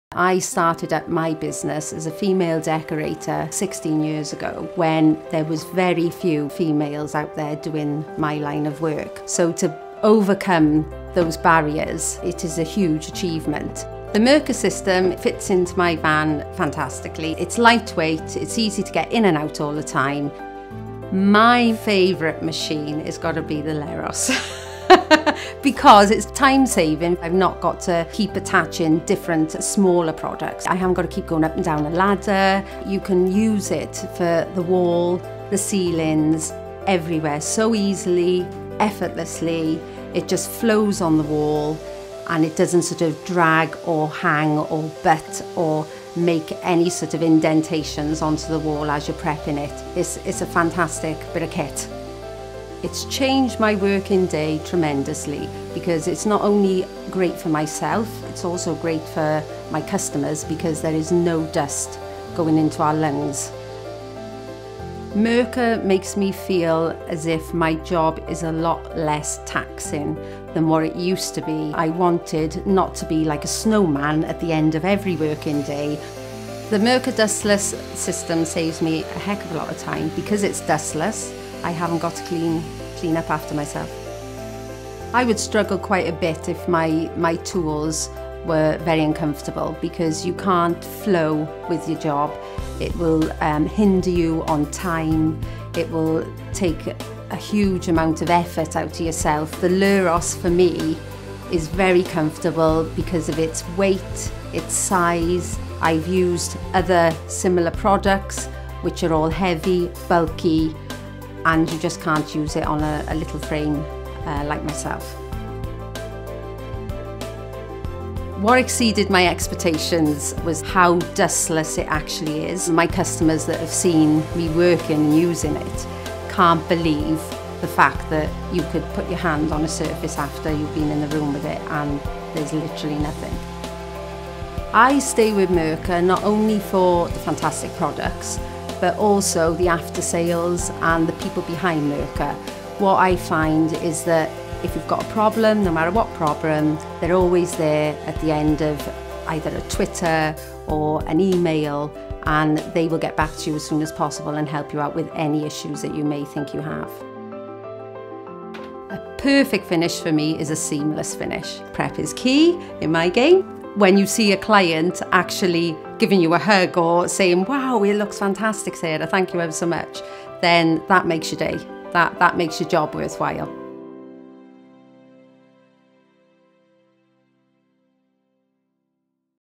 Asiakaskertomus Mirka® LEROS, Paint Pot Ladies
Sarah Hillberg, Paint Pot Ladies, kertoo mitä pölytön hionta merkitsee hänelle.